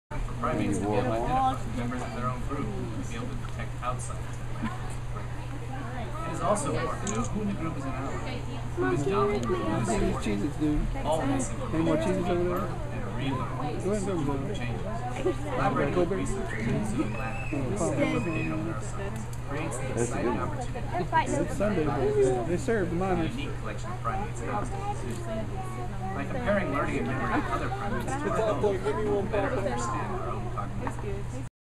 ORANGUTANS BEING AWESOME
A family or orangutans hanging out at Zoo Atlanta. Recorded May 27, 2012.